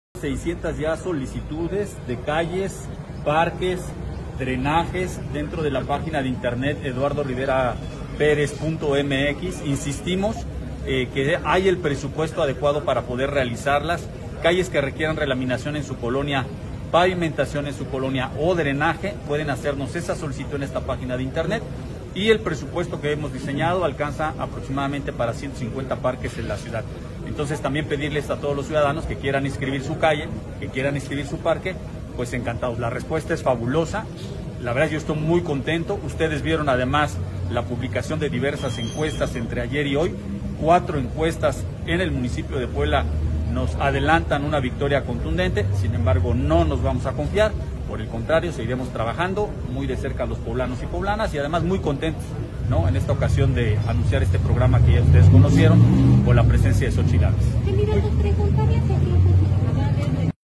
Lalo Rivera ha recibido 600 solicitudes para pavimentar calles y recuperar parques
El candidato común del PAN a la alcaldía de Puebla aseguró que cuatro encuestas le adelantan una victoria contundente.